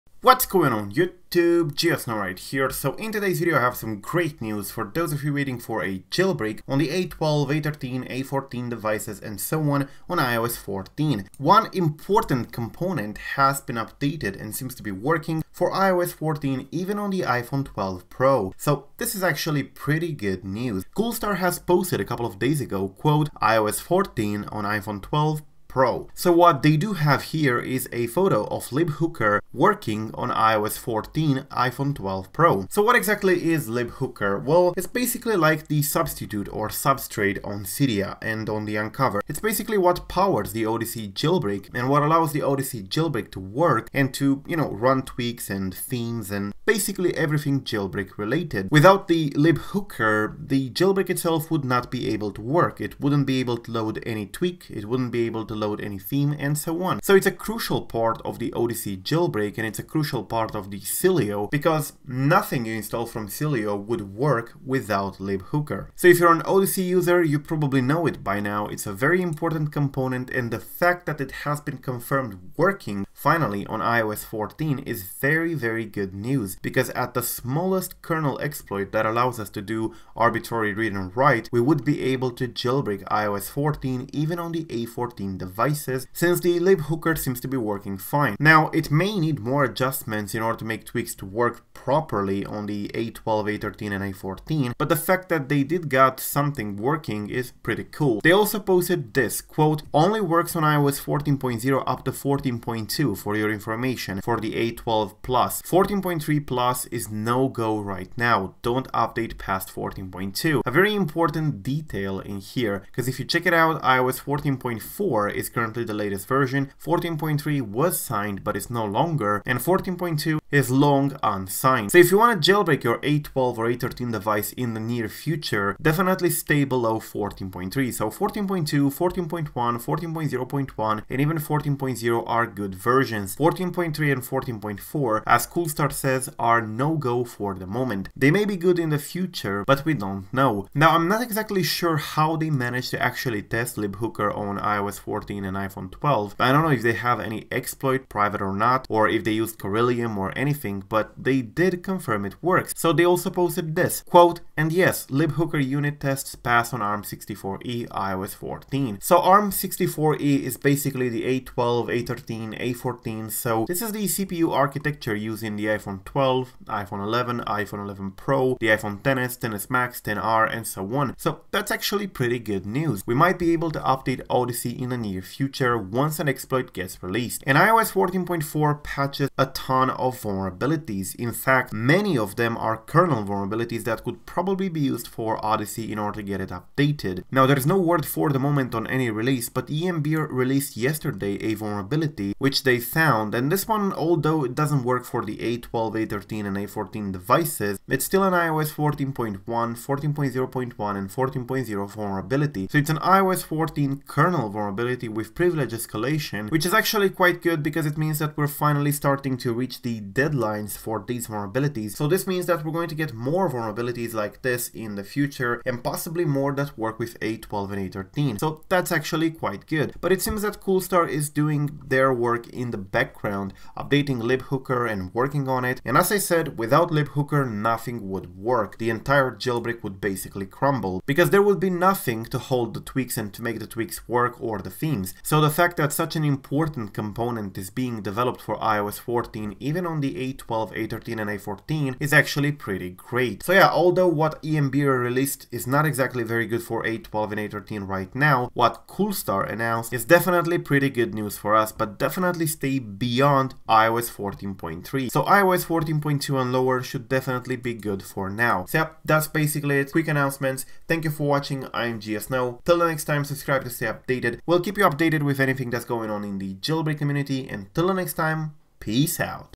iOS 14.2 / 14.1 / 14.0 A13 JAILBREAK Update: Important Odyssey Jailbreak Component WORKS on iOS 14!
In this video, we're discussing the latest Jailbreak news for iOS 14.2 down to iOS 14.0 coming from the Odyssey Team. They have announced that an important component of the Odyssey Jailbreak is now working on iOS 14 even on the A12, A13, and A14 devices, which means that it works on iPhone 12, iPhone 11, iPhone XS, XR, and XS Max as well.

We're talking about LibHooker, which is the Substrate or Substitute equivalent on the Odyssey Jailbreak. It runs the tweaks and allows them to work properly. Every tweak or theme you install depends on LibHooker to be able to load properly on your jailbroken device. Having this component confirmed working on A12 / A13 is great news.

~ GeoSn0w